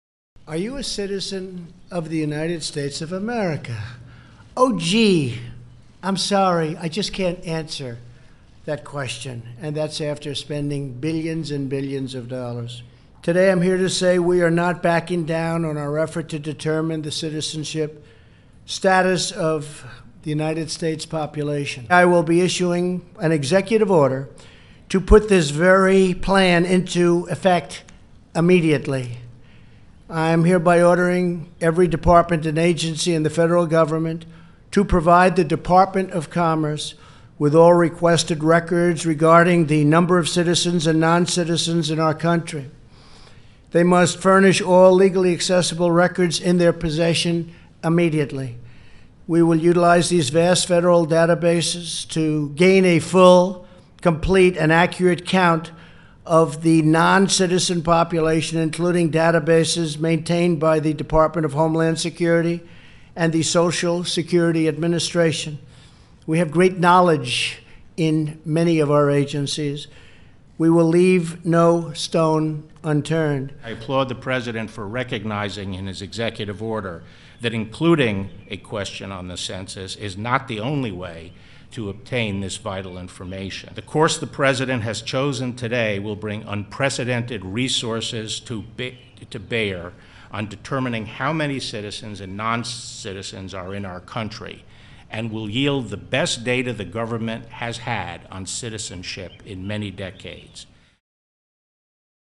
Trump orders federal agencies to hand over citizenship data
Donald Trump abandoned his attempt to add a citizenship question to the 2020 US census but will sign an executive order forcing federal agencies to hand over citizenship data to the commerce department. 'As a result of today's executive order we will be able to ensure the 2020 census generates an accurate count of how many citizens, non-citizens and illegal aliens are in the United States of America. We will leave no stone unturned,' the US president said. The attorney general, William Barr, said including a question on the census was not the only way to obtain this 'vital information'.